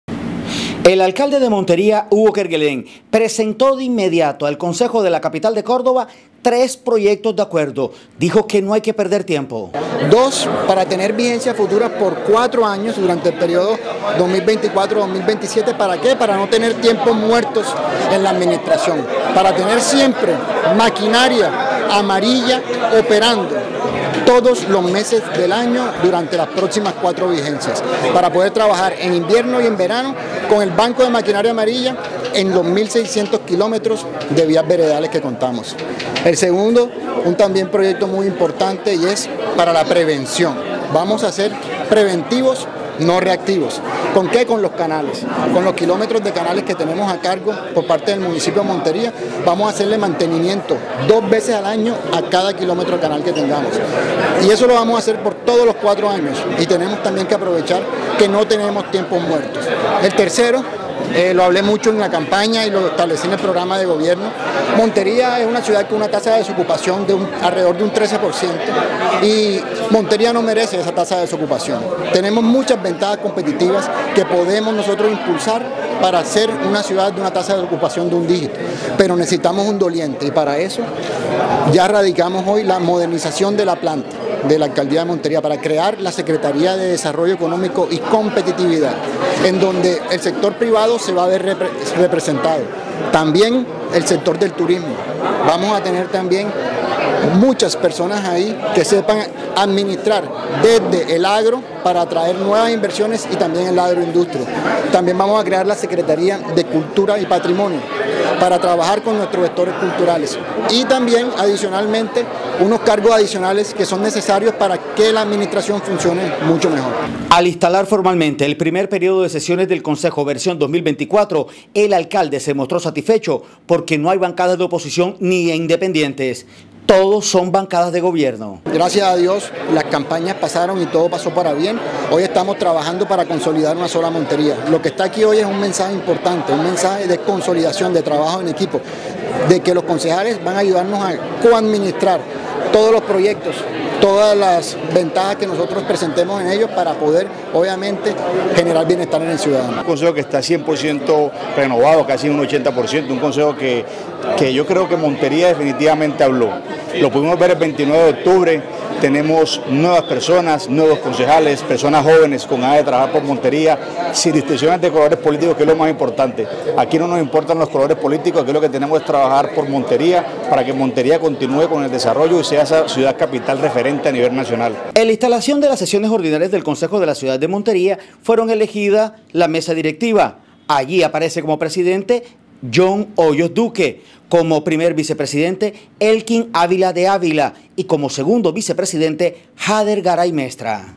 Alcalde de Montería, Hugo Kerguelén, radicó ante el concejo municipal, tres proyectos de acuerdo
Además instaló formalmente el primer periodo de sesiones ordinarias de esa Corporación donde todas las bancadas se declararon de gobierno.